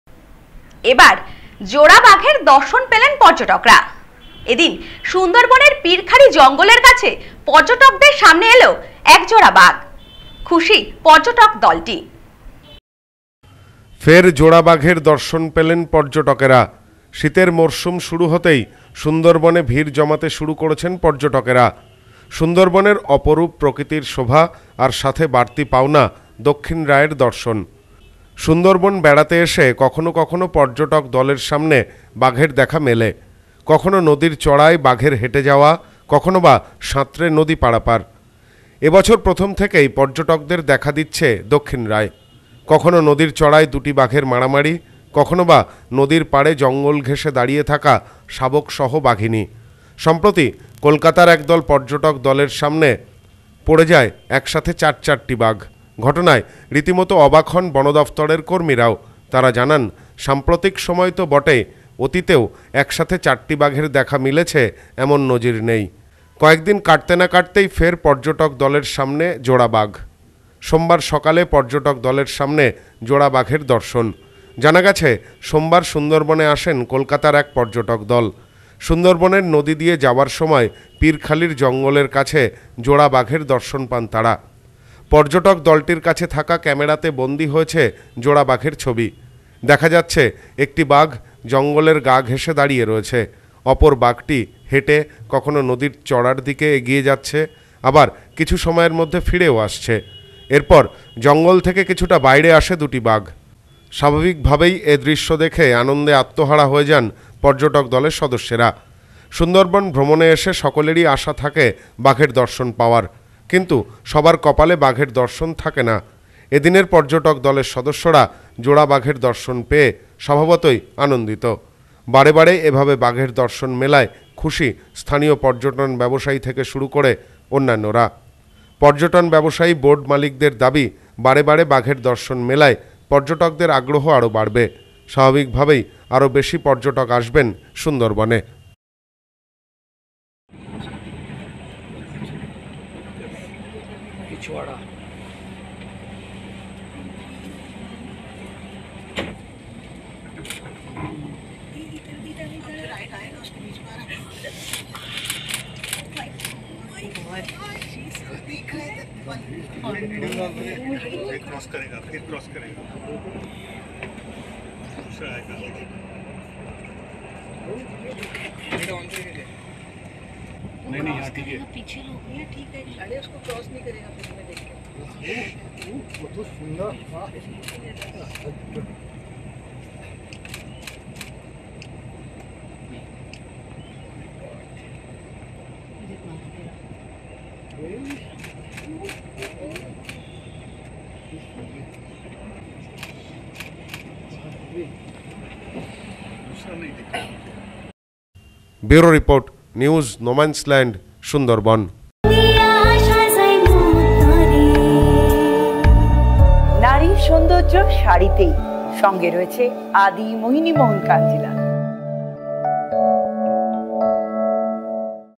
সোমবার সুন্দরবনে জোড়া বাঘের দর্শন পেলেন পর্যটকেরা
এবার জোড়া বাঘের দর্শন পেলেন পর্যটকেরা। এদিন সুন্দরবনের পিরখালি জঙ্গলের কাছে পর্যটকদের সামনে দেখা মেলে এক জোড়া বাঘের। খুশি পর্যটক দলটি।